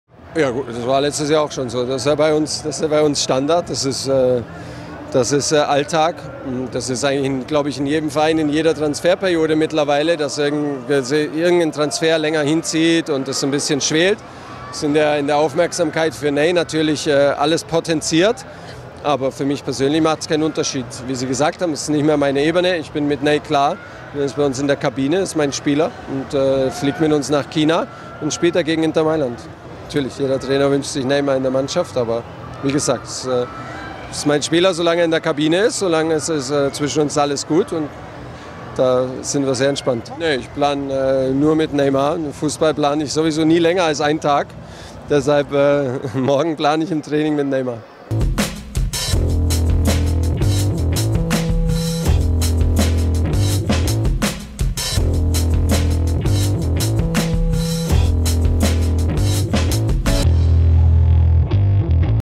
Neymar-Wechsel? Thomas Tuchel klärt auf | SPORT1
PSG-Trainer Thomas Tuchel spricht nach dem Testspiel gegen den 1.FC Nürnberg über die Causa Neymar. Der wechselwillige Brasilianer wird mit ins Trainingslager fliegen.